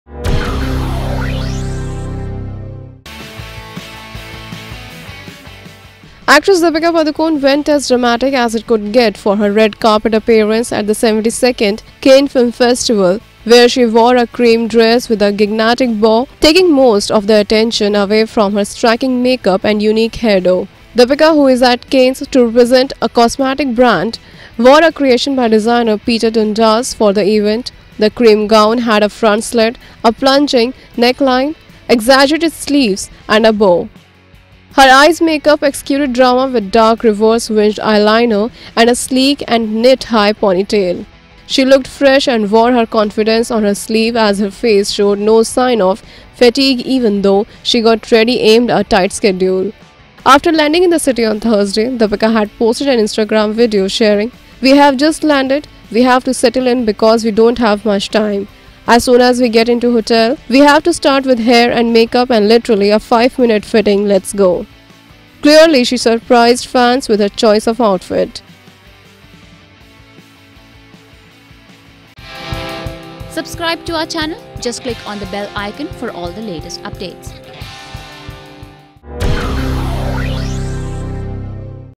Deepika goes dramatic with big BOW at CANNES
Actress Deepika Padukone went as dramatic as it could get for her red carpet appearance at the 72nd Cannes Film Festival, where she wore a cream dress with a gigantic bow taking most of the attention away from her striking make-up and unique hairdo. Deepika, who is at Cannes to represent a cosmetics brand, wore a creation by designer Peter Dundas for the event. The cream gown had a front slit, a plunging neckline, exaggerated sleeves and a bow.